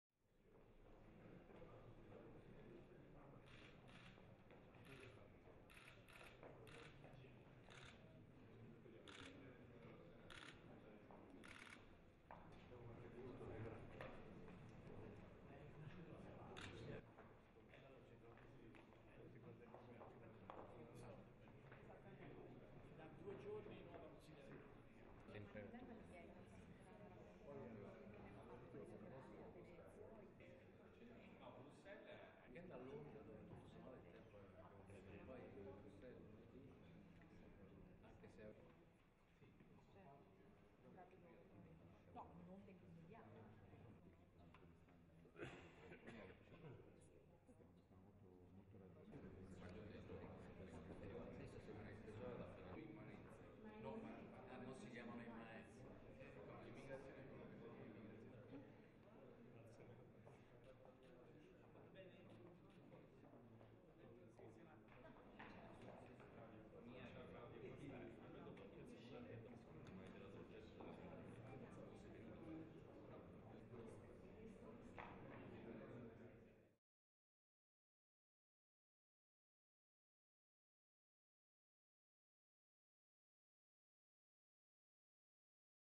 incontro del Presidente Mattarella con il Presidente del Consiglio e membri del governo
incontro del Presidente della Repubblica Sergio Mattarella con il Presidente del Consiglio dei Ministri e altri membri del Governo in occasione del prossimo Consiglio Europeo